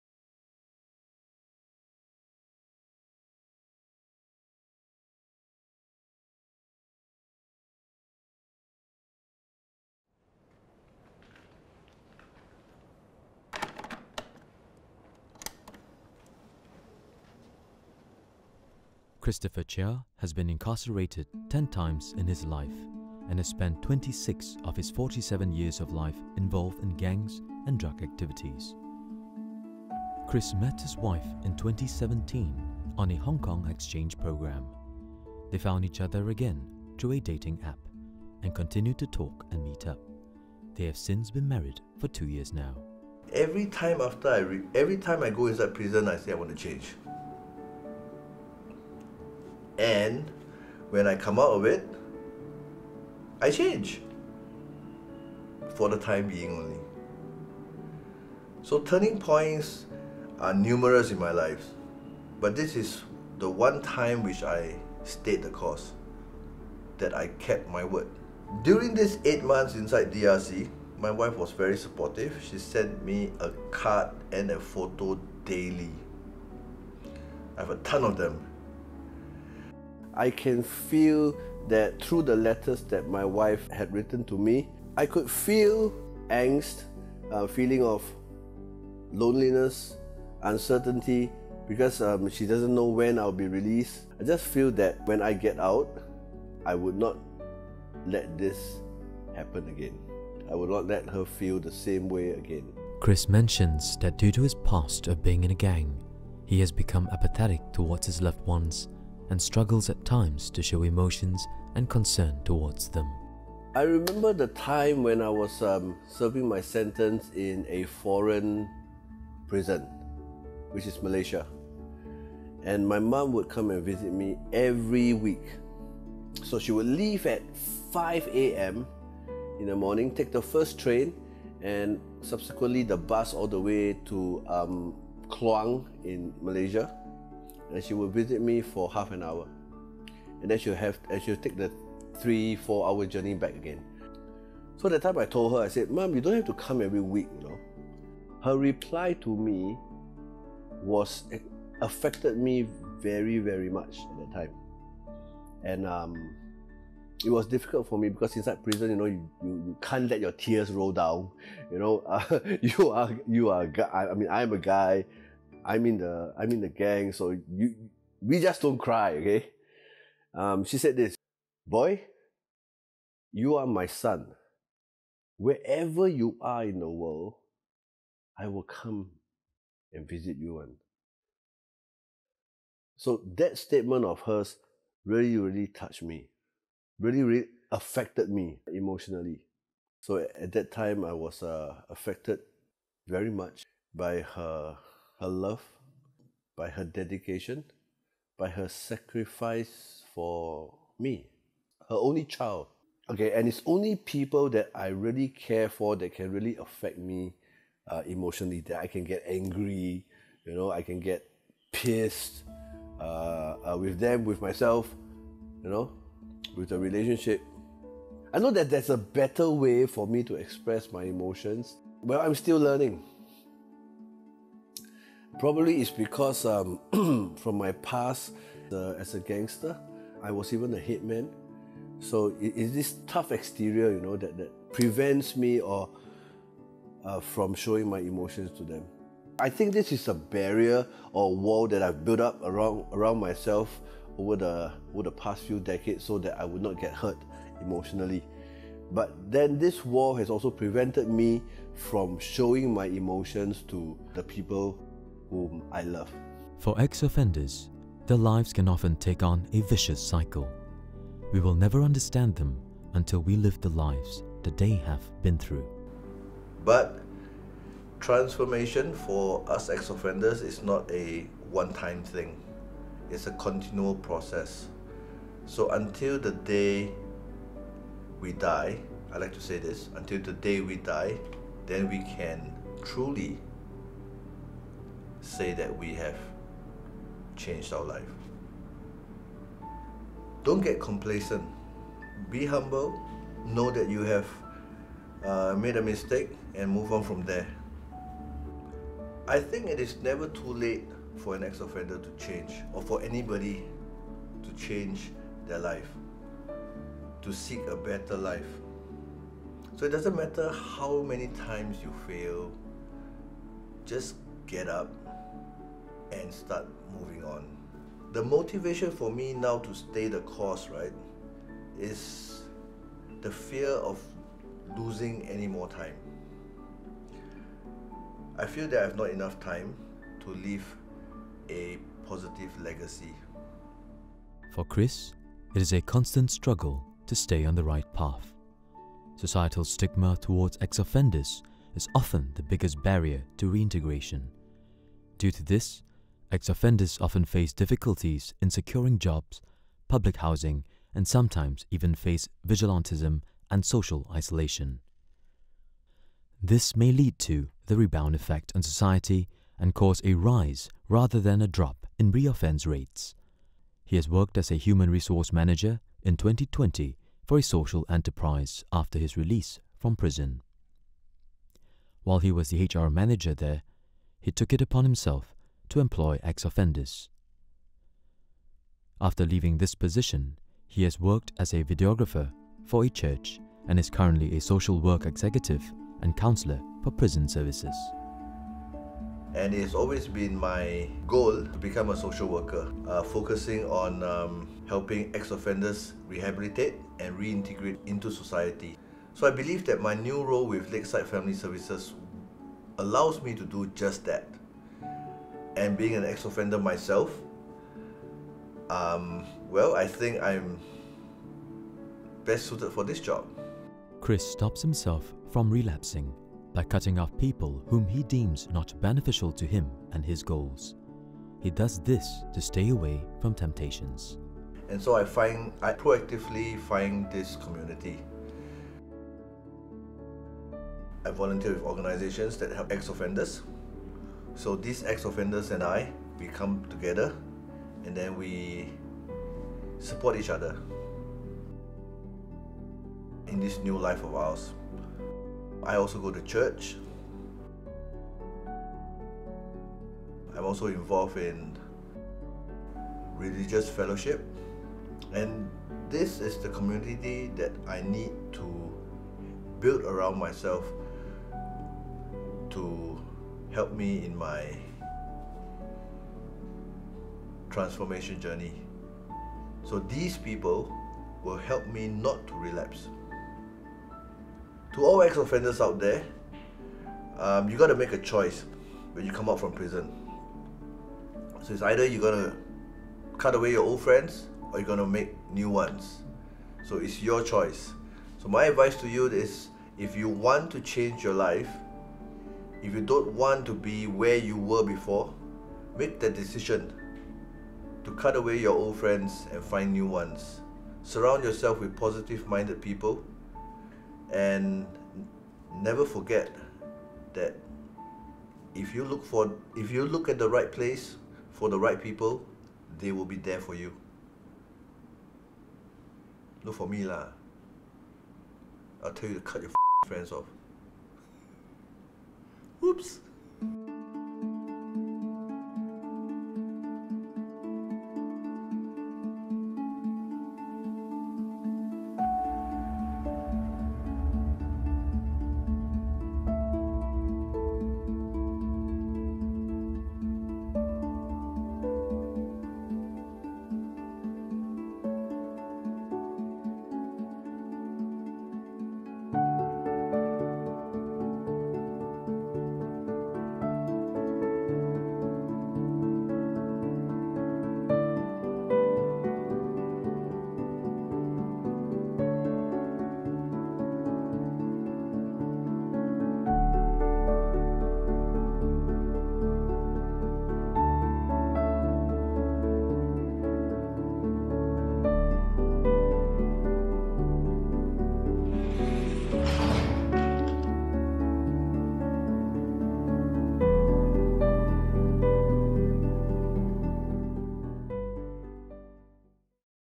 The Second Prison - Documentary
Year 2 documentary project - Temasek Polytechnic

"Chris, an ex-convict, shares about his life in prison and his struggles to integrate back into society and become a success story for other ex-convicts."

Crew:
Director – Charlene Jody Lim
Producer – Rachel Wong Si Ying
Assistant Producer – Lim Hong Jun
Assistant Director – Benjamin Lee Sze Yong
Camera Operator – Jovan Choo Kun Hao, Ng Jordan
Sound Recordist – Yak Chin Hue
Editor – Lee Wo Xuan